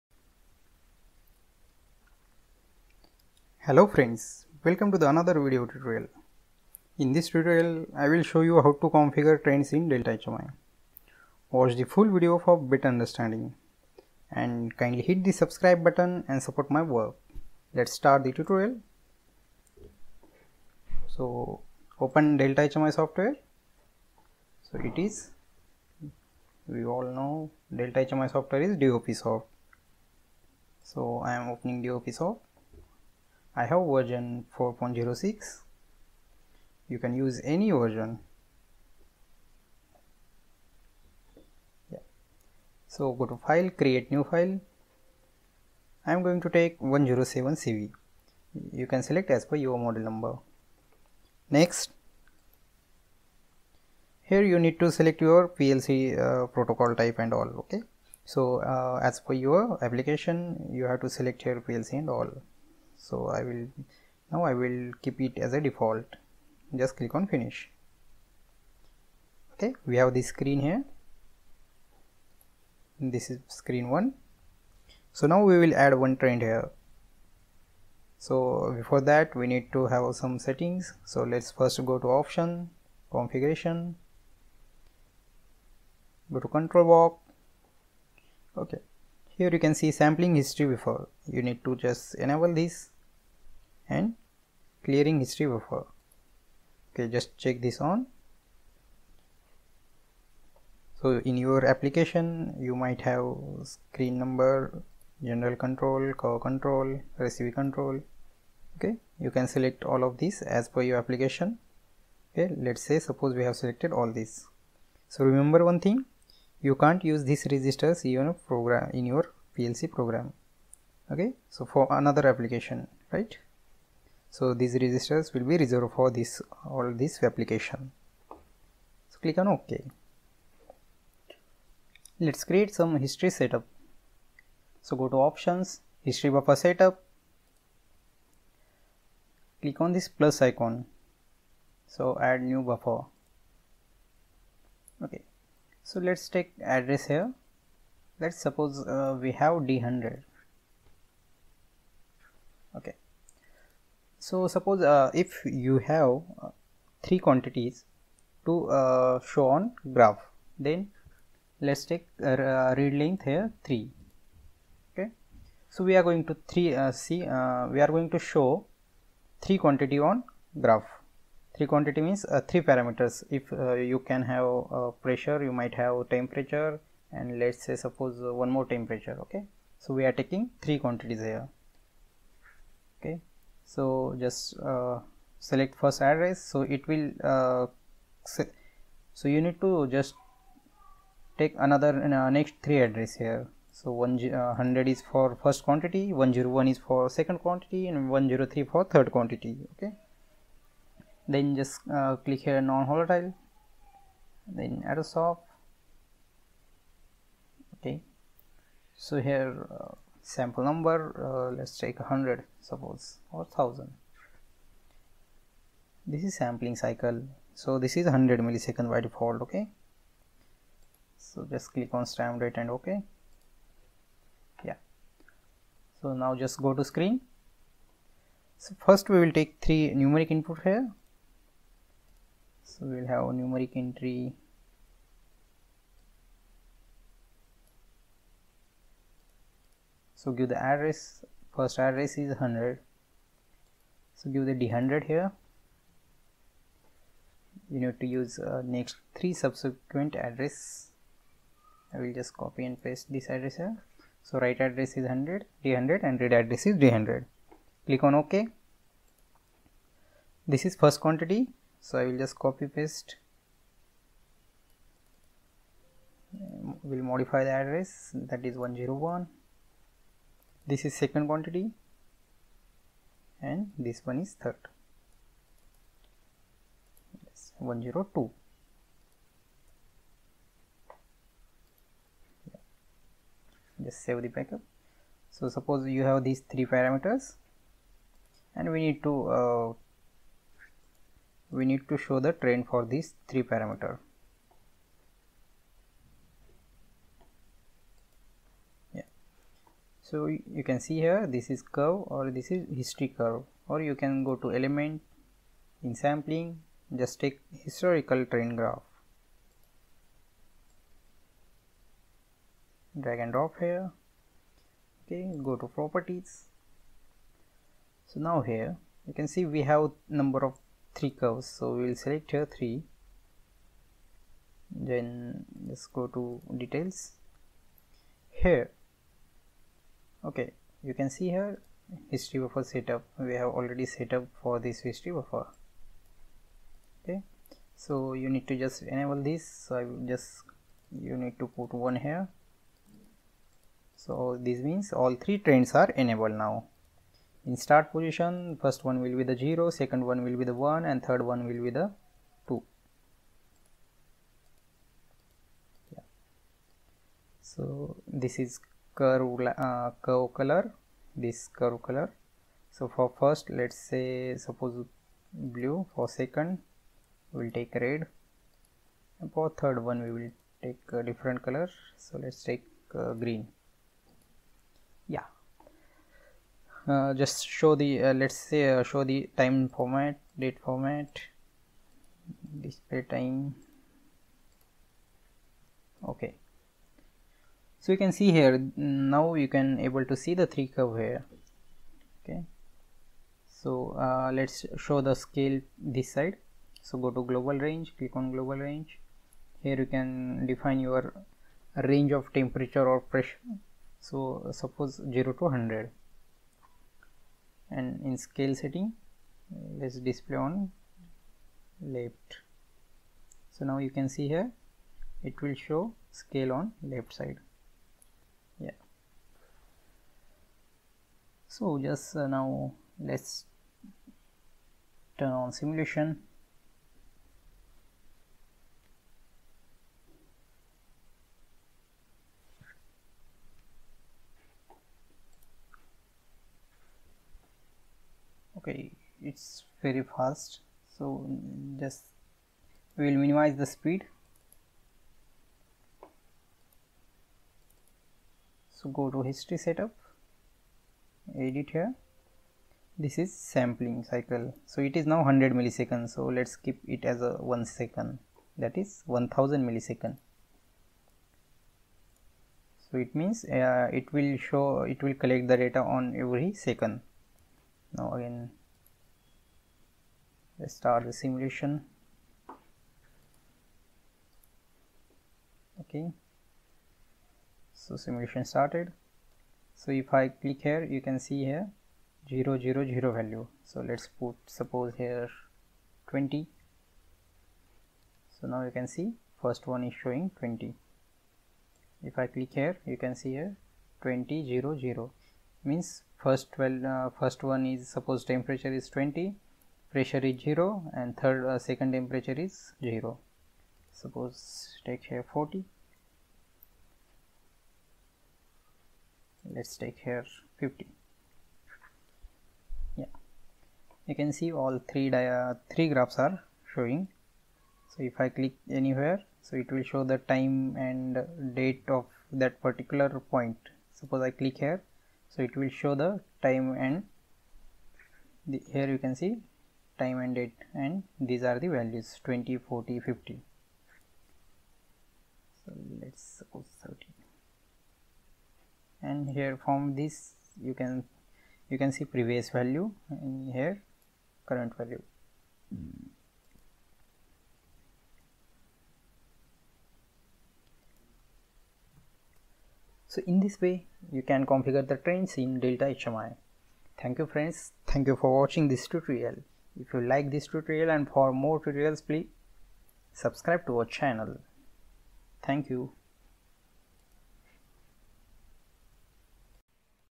Trends (Graph) In Delta HMI 📈 | English
Hello Friends,
This video is complete guide for configuration of trends in delta HMI.
Following things you will understand,
1. How to configure trends in delta HMI
2. How to creat history buffer in delta HMI
3. Multiple parameter graph
4. How to show Scalling in trends